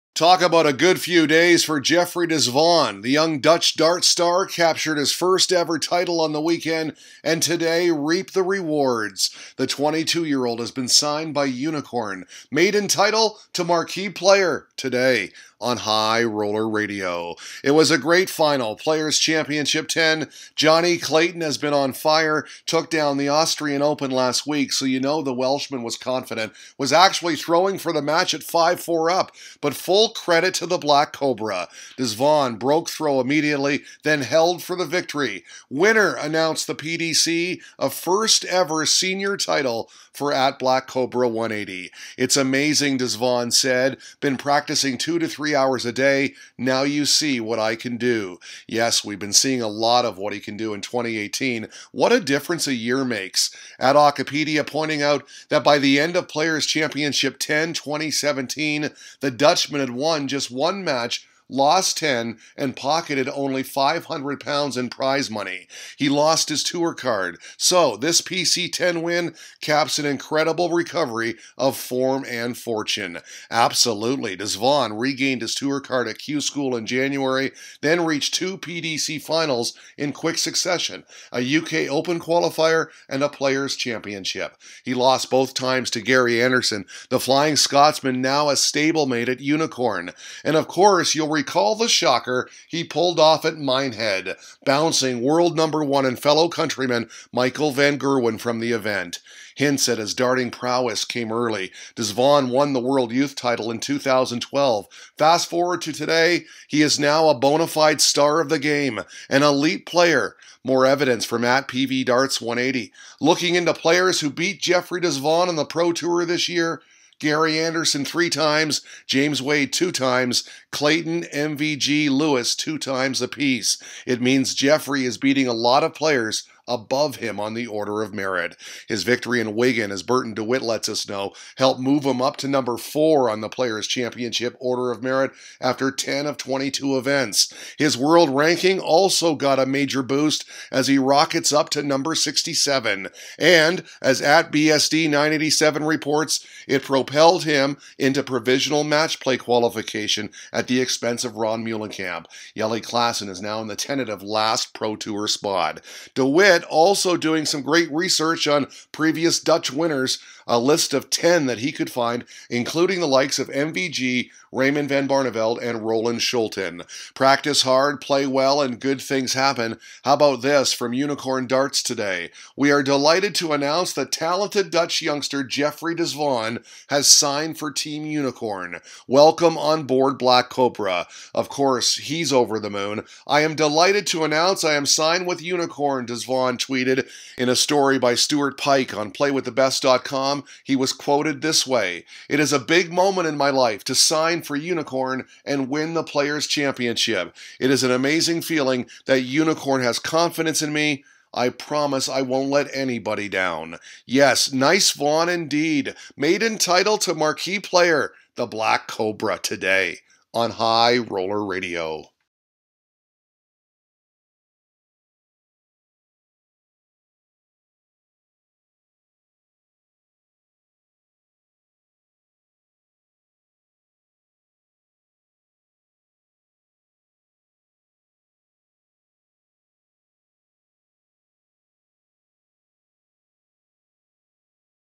Maiden Title to Marquis Player @UnicornDarts! Jeffrey de Zwaan Signed by TeamUnicorn
It's been a good week for the Black Cobra! Wins PC10 for first ever senior PDC title & now today is signed by TeamUnicorn.